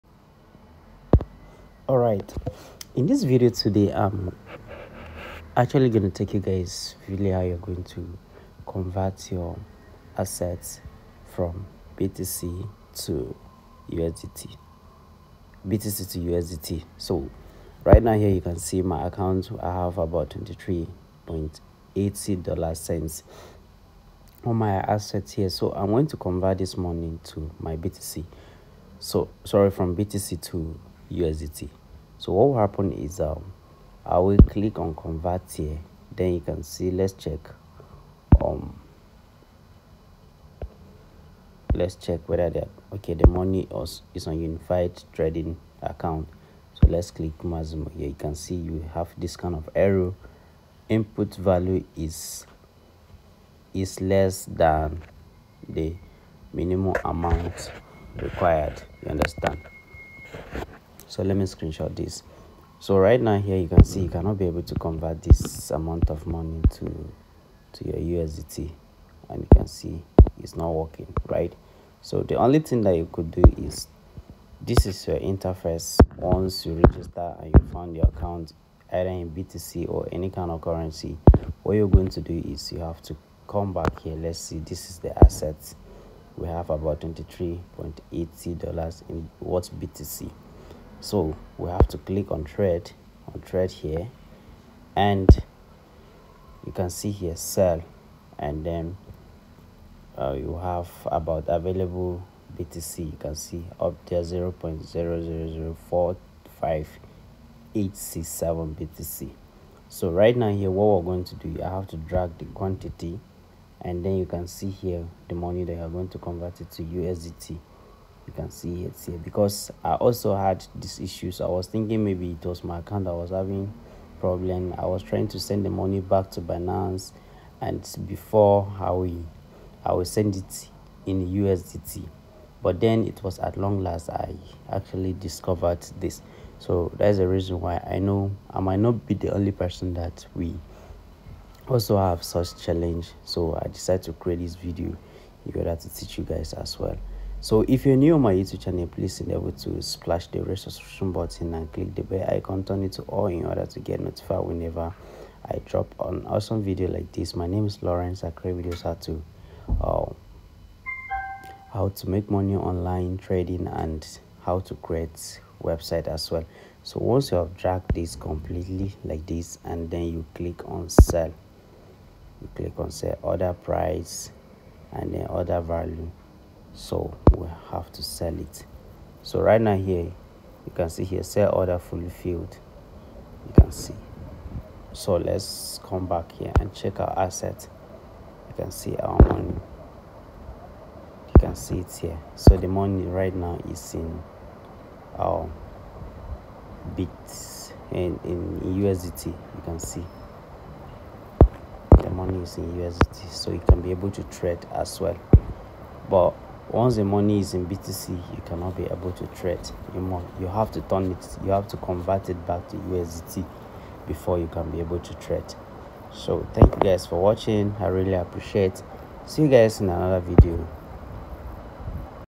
How To Convert BTC to USDT On Bybit (EASY)
I will show you how to convert BTC to usdt on bybit in this video. this is the updated version of how to convert on bybit it is no longer called exchanging like it was in 2022 but it converts. watch the video